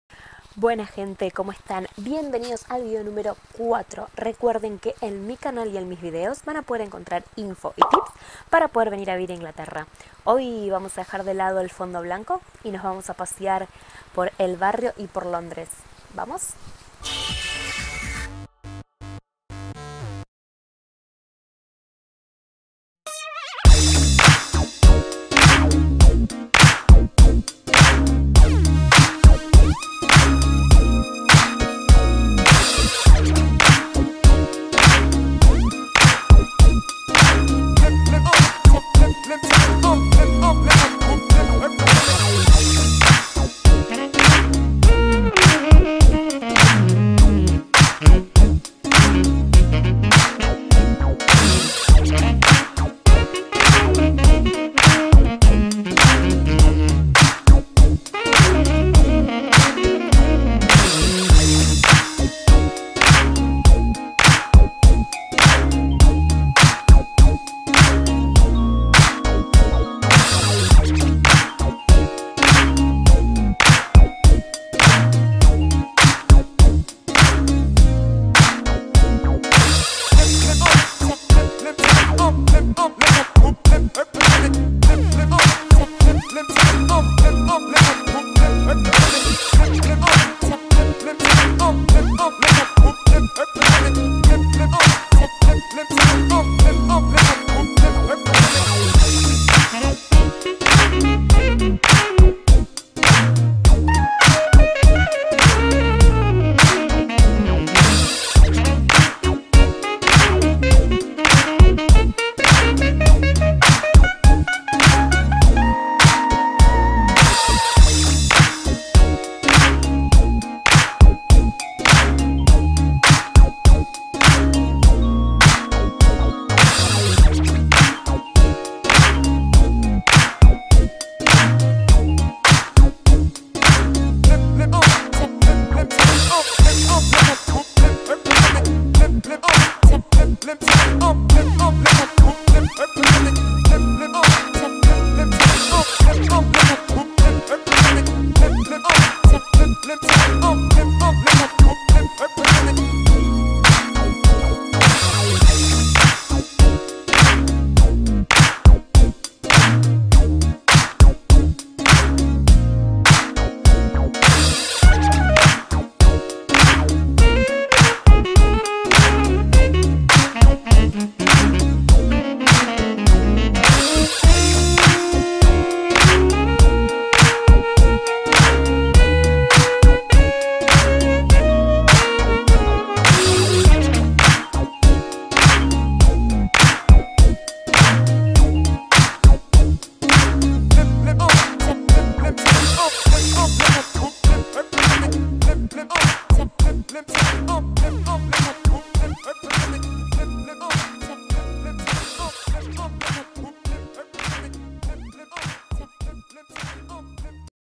MINI paseo por Londres | Argie In London
Buenas Gente 🤙🏼.
Acá van a poder encontrar info y tips para venir a vivir a UK.

+ Gracias por mirar mi video! ♥

Anduvimos visitando:
+ Westmister Abbey
+ London Eye
+ Big Ben
+ Notting Hill
+ Piccadilly Circus
+ Mi Barrio: Cricklewood